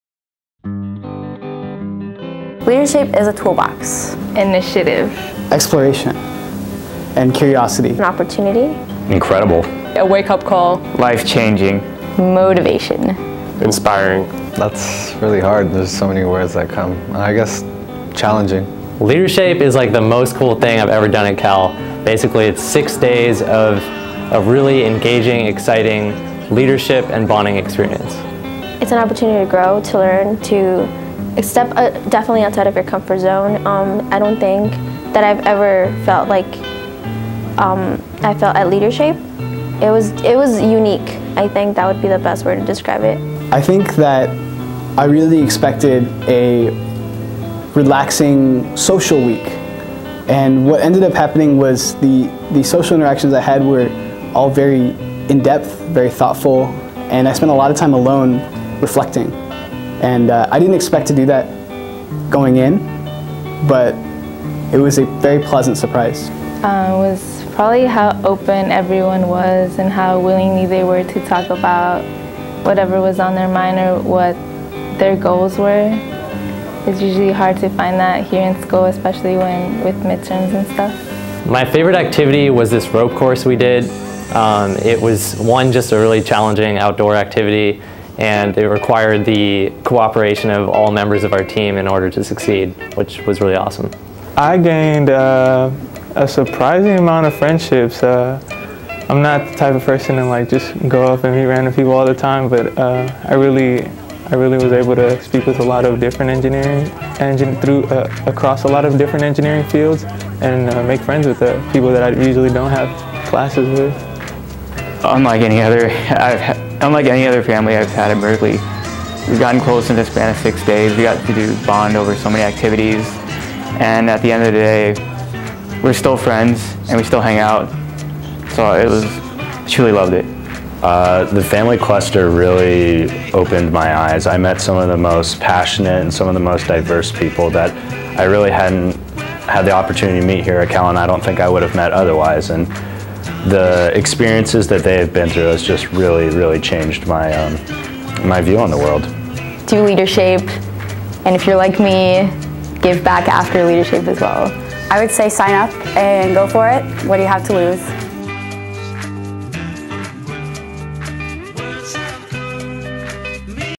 Berkeley LeaderShape 2015
Berkeley Engineering students explain why they enjoyed the LeaderShape Institute and why more students should take part.